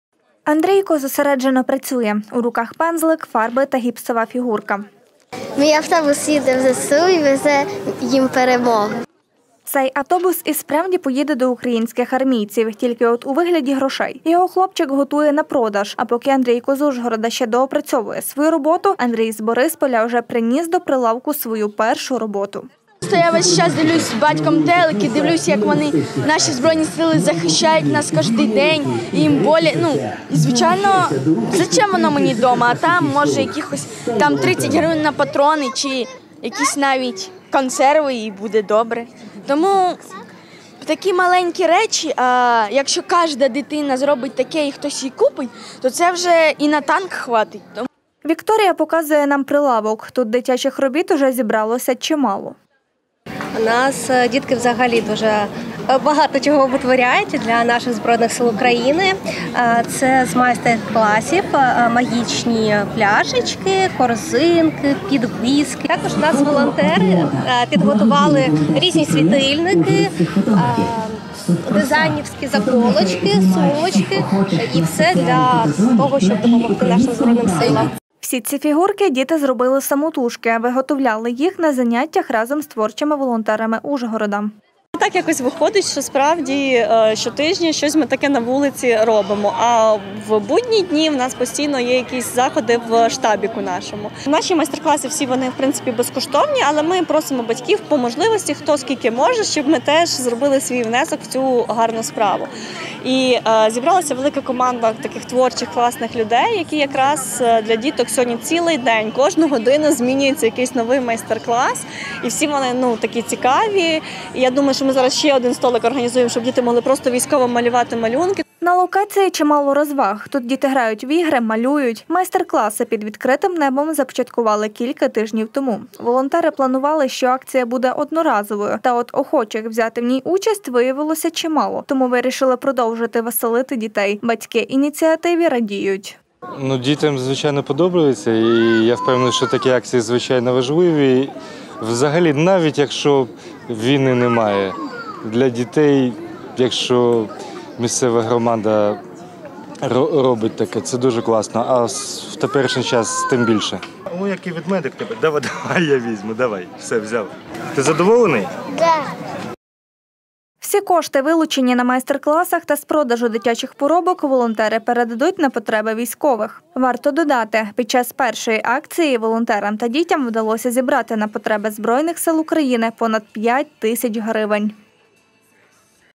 Для ЗСУ від дітей. Благодіний ярмарок
В Ужгороді вкотре пройшов благодійний ярмарок. Творчі волонтери Ужгорода разом із дітьми виготовляли цікаві аксесуари та одразу продавали їх. Все аби зібрати кошти на потреби ЗСУ. Дітей зібралося чимало, а от які вироби виготовили цього тижня. Андрійко зосереджено працює. В руках пензлик, фарби та гіпсова фігурка. Цей автобус і справді поїде в до українських армійців. Тільки от у вигляді грошей. Його хлопчик готує на продаж.  А поки Андрійко з Ужгорода ще допрацьовує свою роботу, Андрій з Борисполя, вже приніс до прилавку свою першу роботу. Вікторія показує нам прилавок. Тут дитячих робіт вже зібралося чимало. Всі ці фігурки діти зробили самотужки. Виготовляли  їх на заняттях разом з творчими волонтерами Ужгорода. На локації чимало розваг. Тут діти грають у ігри, малюють. Майстер класи під відкритим небом започаткували кілька тижнів тому. Волонтери планували, що акція буде одноразовою, та от охочих взяти в ній участь виявилося чимало, тому вирішили продовжити веселити дітей. Батьки ініціативі радіють. Всі кошти вилучені на майстре класах, та з продажу дитячих поробок волонтери передають на потреби військовим. Варто додати під час першої акції волонтера та дітям вдалося зібрати на потреби ЗСУ понад 5 тисяч гривень.